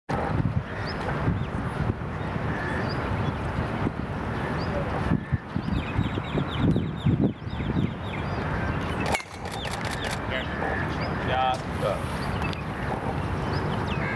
Muni He of China
Omega Dubai Ladies Masters, Emirates Golf Club, Dubai, UAE. 7-10 December 2016